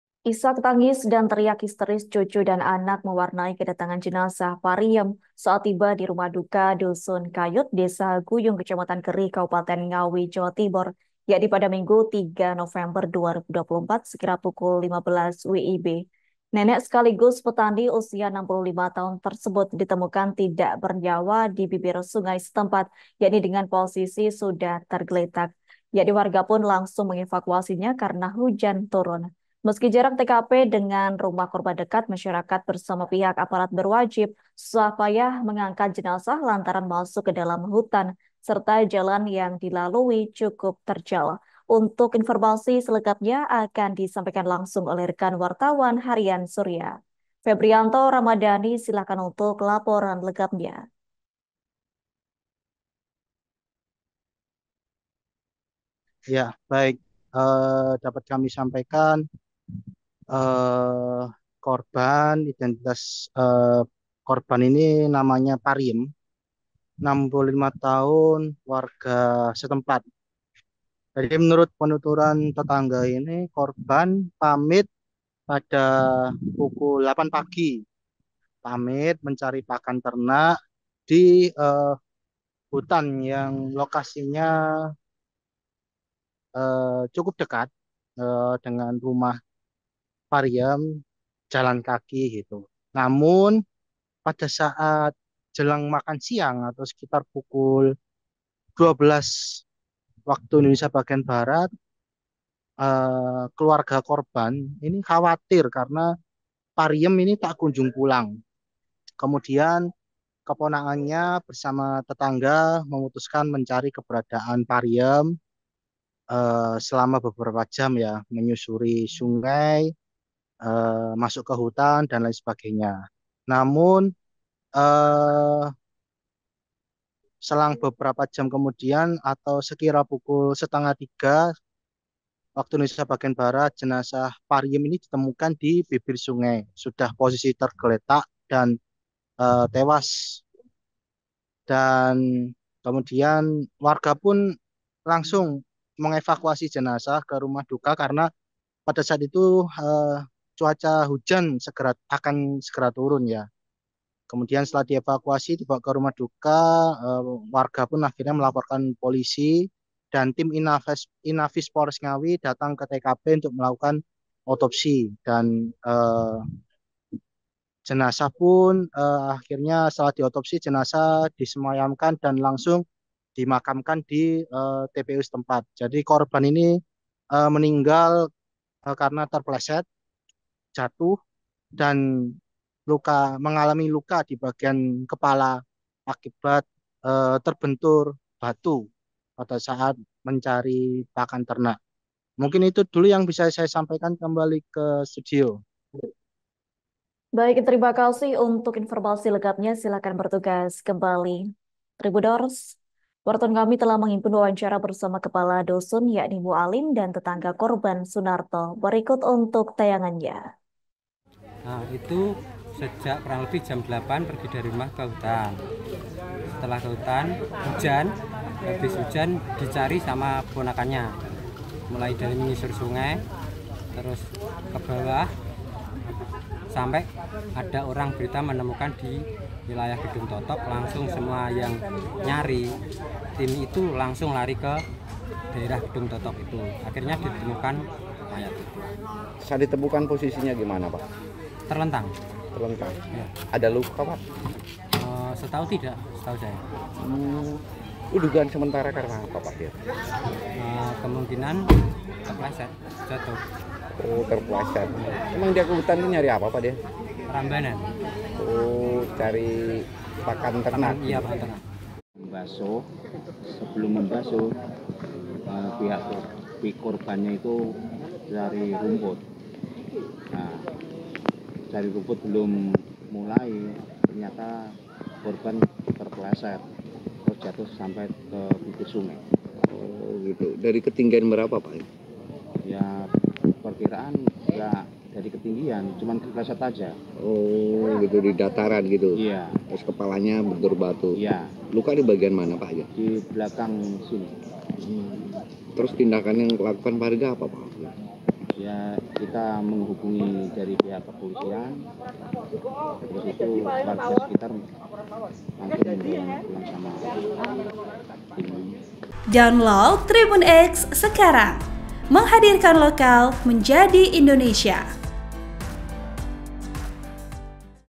Diduga Jatuh Terpeleset, Nenek Ditemukan Tewas Tergeletak di Dekat Jurang Desa Guyung Ngawi
Laporan wartawan, Harian Surya - Febrianto Ramadani
TRIBUN-VIDEO.COM - Isak tangis dan teriak histeris cucu dan anak, warnai kedatangan jenazah Pariyem, saat tiba di rumah duka, Dusun Kayut, Desa Guyung, Kecamatan Gerih, Kabupaten Ngawi, Jawa Timur, Minggu (3/11/2024) sekira pukul 15.00 WIB.

Nenek sekaligus petani usia 65 tahun tersebut, ditemukan tidak bernyawa di bibir sungai setempat, dengan posisi sudah tergeletak. Warga pun langsung mengevakuasinya karena hujan segera turun.

Meski jarak TKP dengan rumah korban dekat, masyarakat bersama pihak aparat berwajib, susah payah mengangkat jenazah, lantaran masuk ke dalam hutan, serta jalan yang dilalui cukup terjal. (*)

Program: Live Update
Host: Adila Ulfa Muna Risna
Editor Video: Ika Vidya Lestari
Uploader: Dyah Ayu Ambarwati